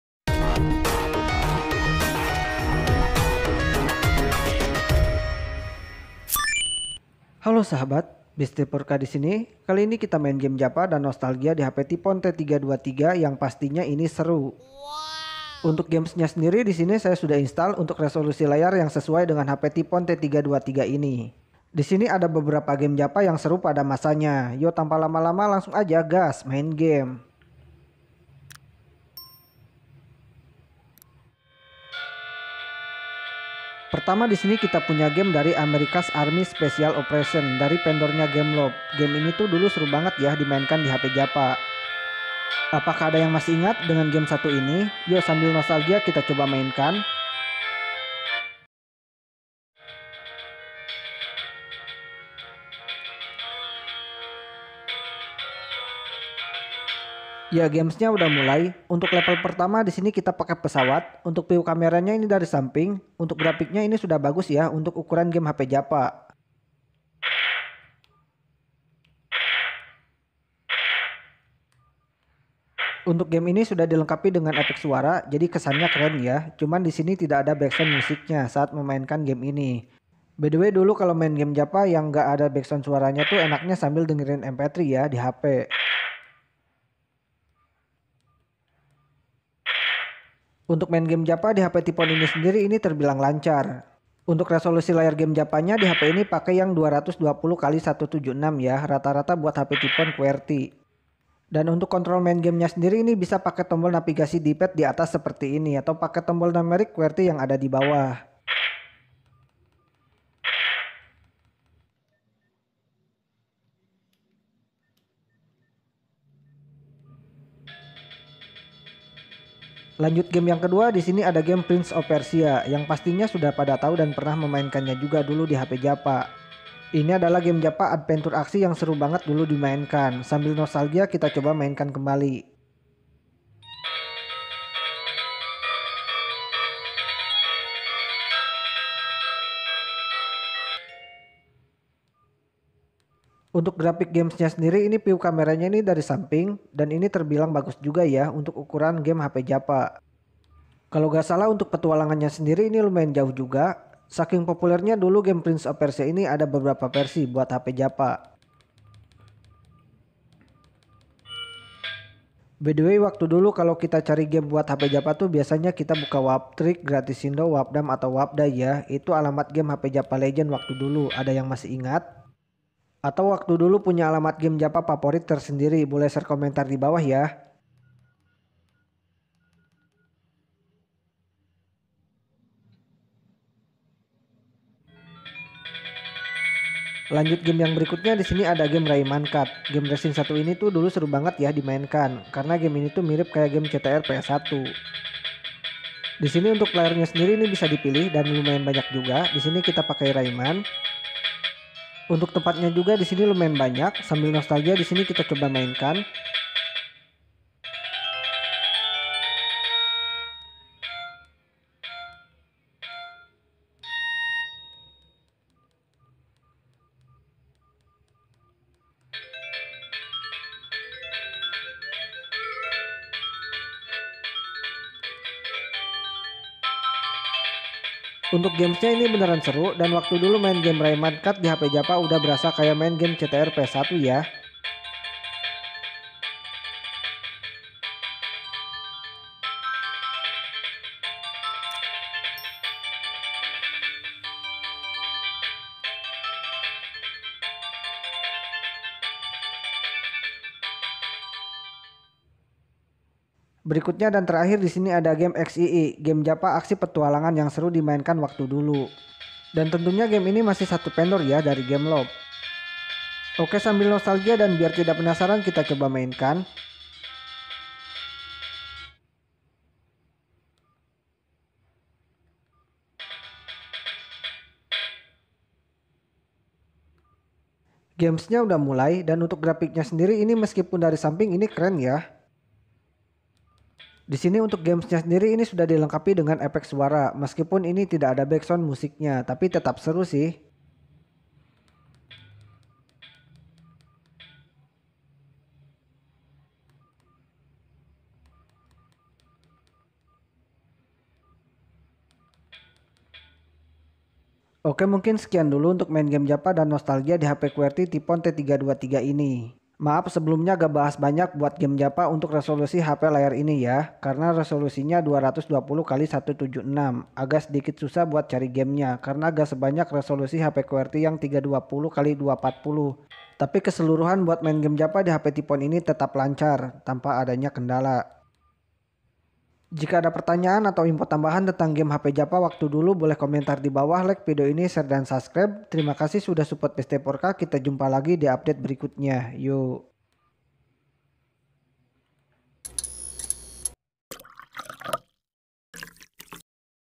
MAIN GAME JAVA GAMELOFT SERU ! - Nostalgia Hp Qwerty Ti.Phone T323
Hallo Kali Ini Kita Main Game Java Gameloft Seru Pada Masanya Di Hp Tiphone T323 Game Yang Pastinya Bikin Rindu Nostalgia Masa Dulu Langsung Saja.

LINK PEMBELIAN
Hp Tiphone T323 :